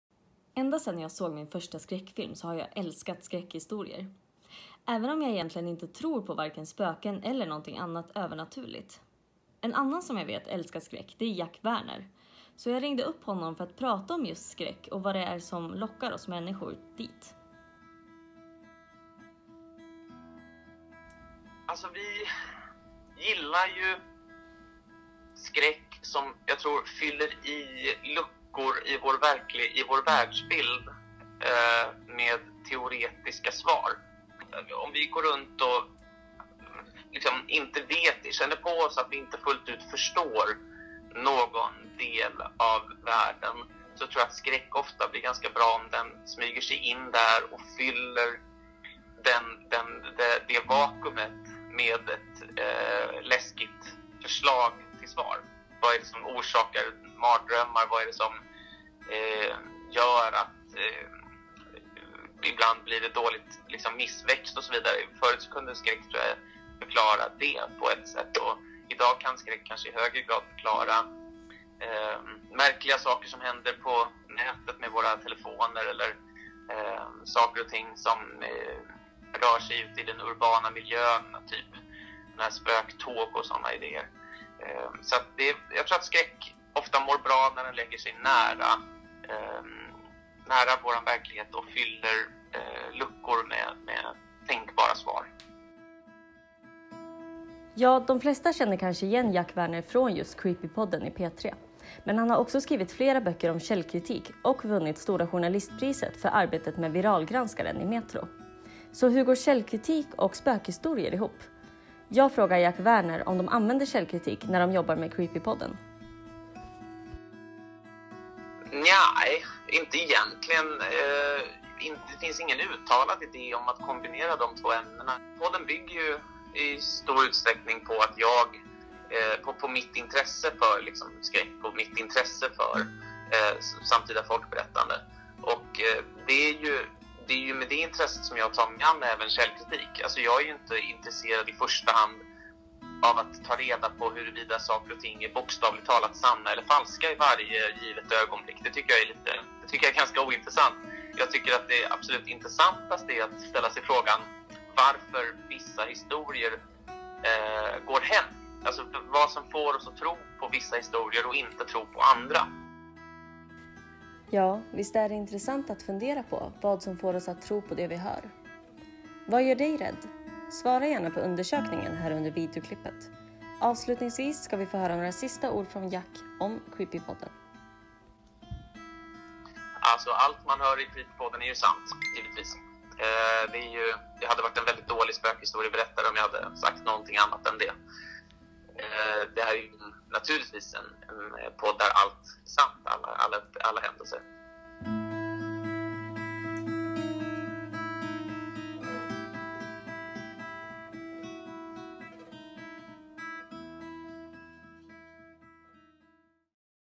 Läget intervjuar Jack Werner
Jack Werners podd Creepypodden är en av de mest lyssnade i Sverige. Läget ringde upp Jack för att prata skräck och varför vi gilla det. Vi frågar också om allt som sägs i Creepypodden är sant.